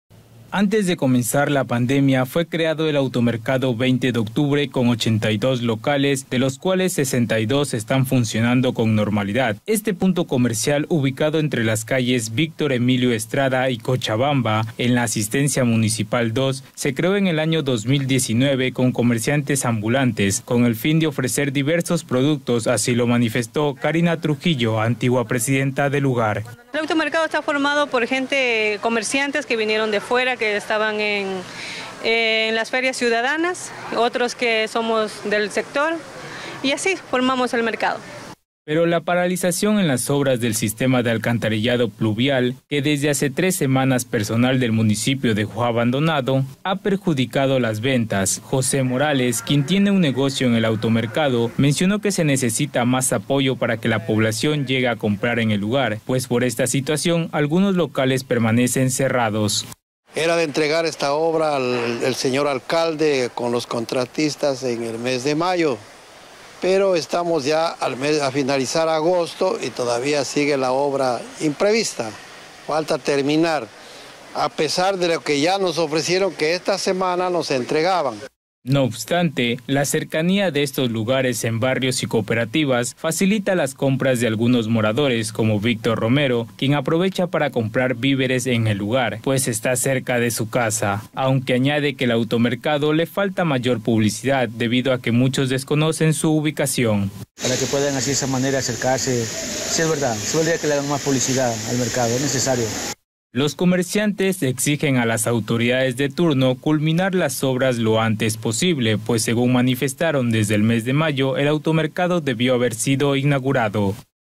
El automercado 20 de Octubre aún espera ser inaugurado | Noticias | Zaracay TV
Los comerciantes del automercado 20 de Octubre siguen esperando que culmine la construcción del sistema de alcantarillado pluvial, pues ha perjudicado las ventas de quienes tienen sus locales en el lugar. De 82 puestos, 20 están cerrados por esta situación.